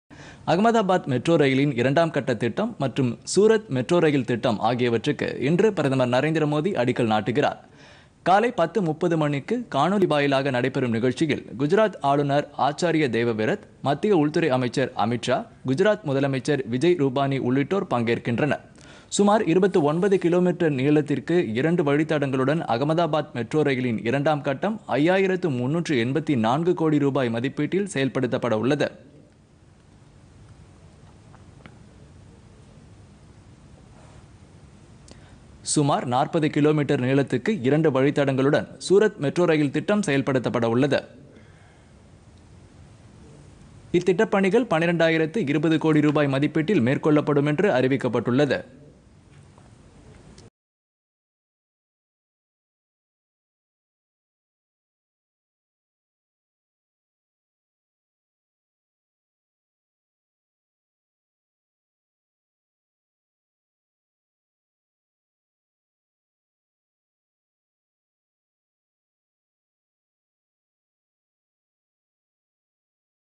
மெட்ரோ ரயில் திட்டத்திற்கு பிரதமர் அடிக்கல்
அகமதாபாத் மற்றும் சூரத் மெட்ரோ ரயில் திட்டங்களுக்கு  இன்று காலை அடிக்கல்  நாட்டுகிறார் பிரதமர் நரேந்திர மோதி